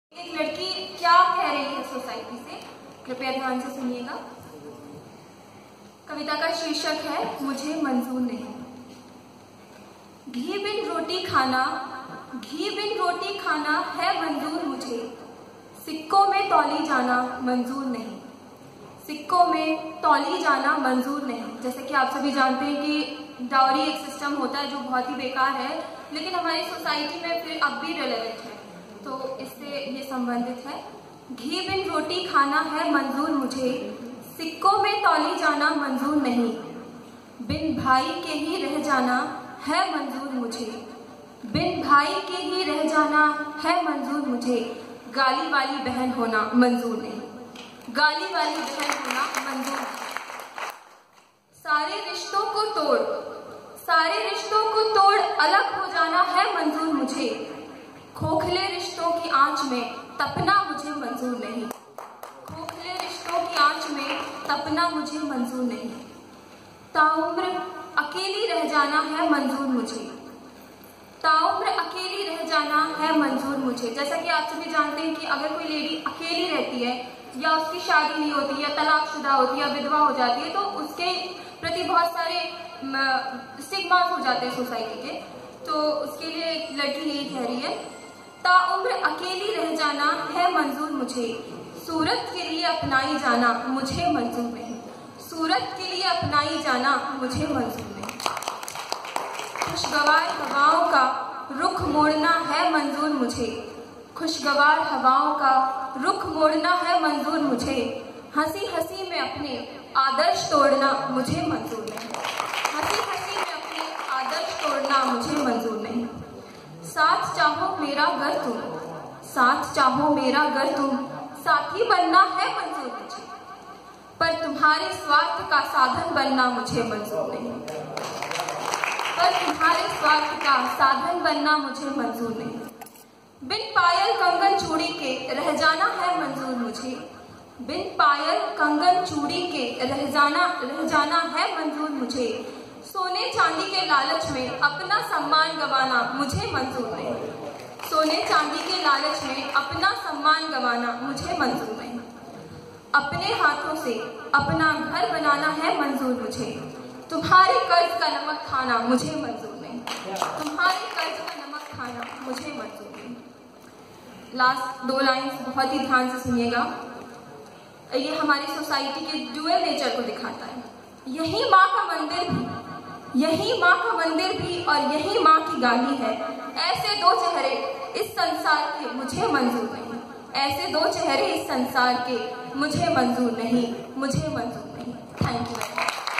"मुझे मंज़ूर नहीं"
A self composed poem of mine presented during Training at Central bank officers training college Bhopal.
(Amiraj Dubey CBI AFO D/o Shri Rajesh Kumar Dubey)